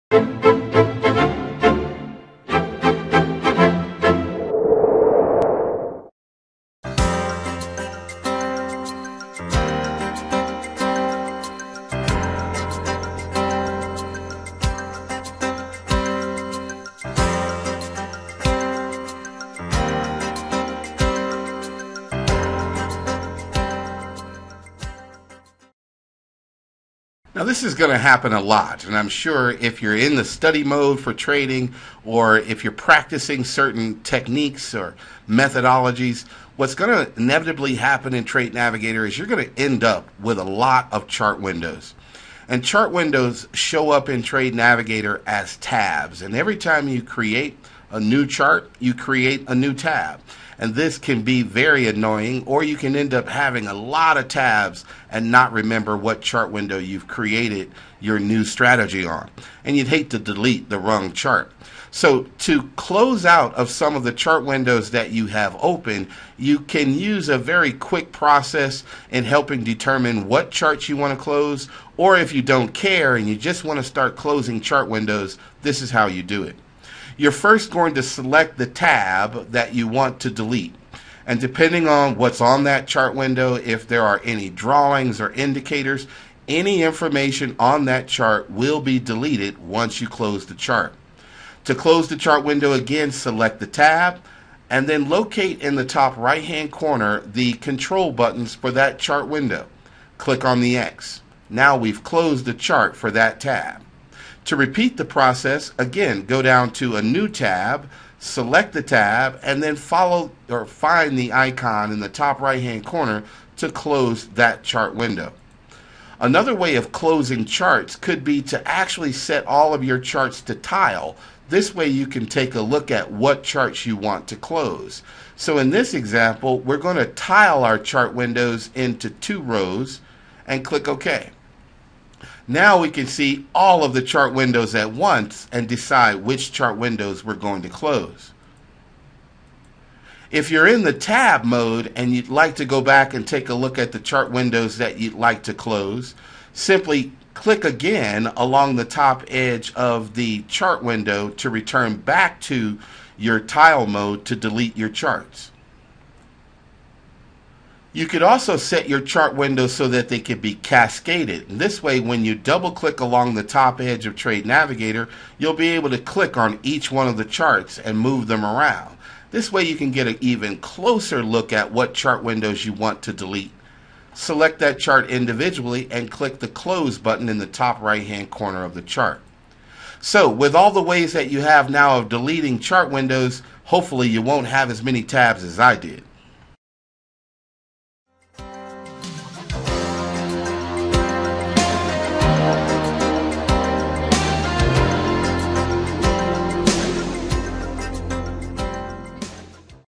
Session -2 how to close Trade Navigator chart windows.[Beginners]
Learn how to close Trade Navigator chart windows. To keep Your Trade navigator organized to be more efficient in your trading .Close any chart windows not being used for ultimate trade navigator performance.